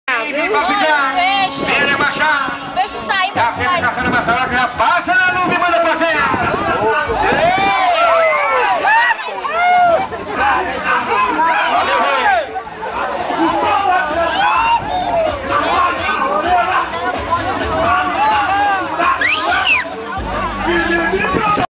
Vaquejada video cassetada - Itapipoca-CE
Cassetada na vaquejada do Parque Luiz Eduardo Itapipoca-CE Data: 21/12/2008. Foi a Mãe que Gravou (Ana Lúcia)...erer!